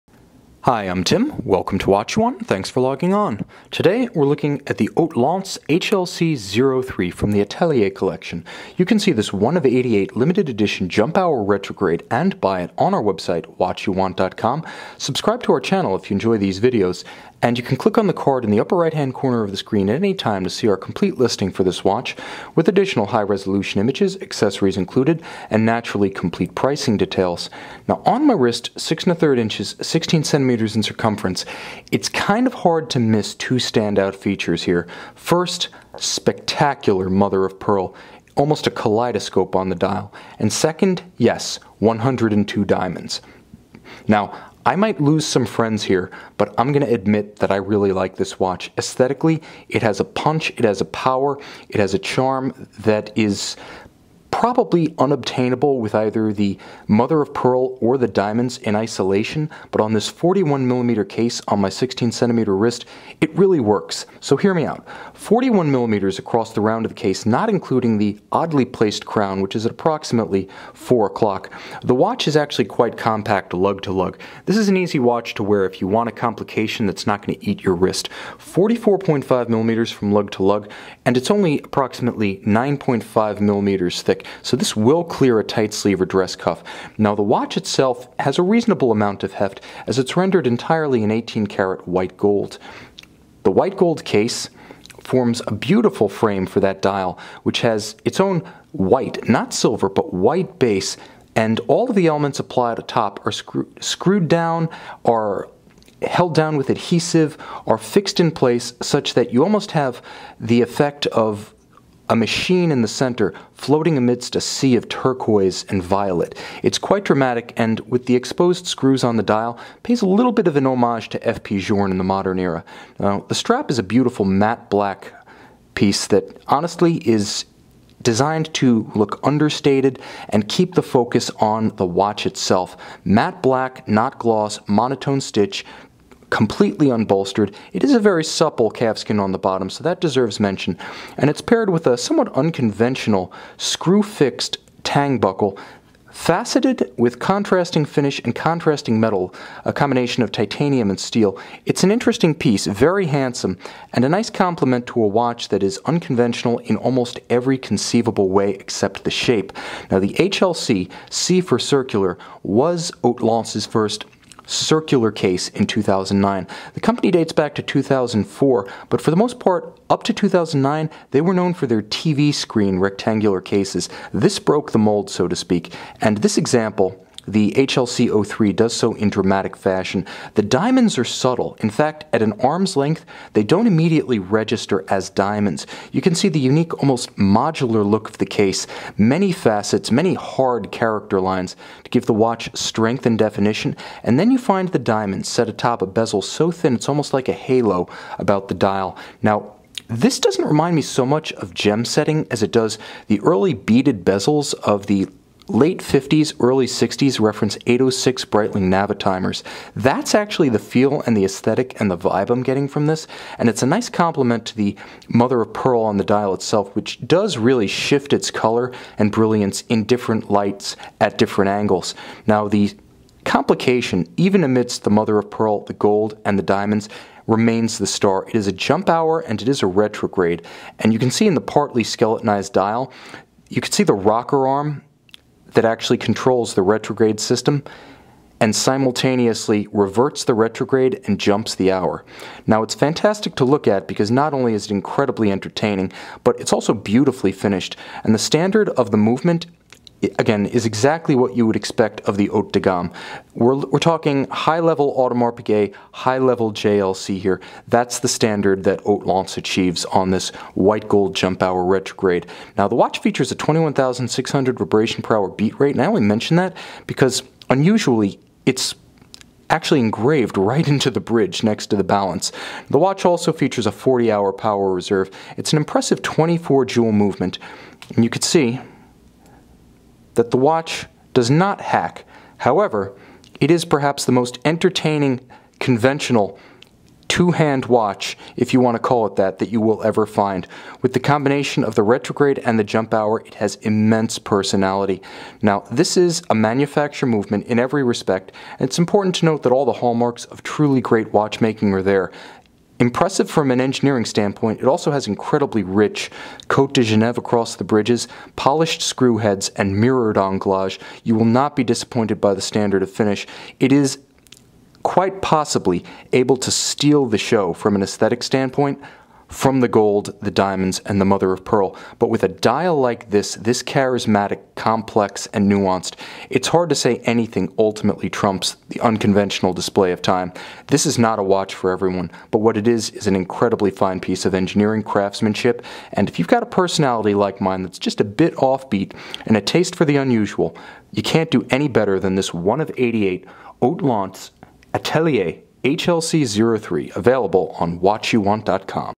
Hautlence HLC 03 Atelier Luxury Watch Review
See this 1-of-88 limited edition, 44mm 18K white gold Hautlence HLC 03 Atelier in high-resolution images on our website,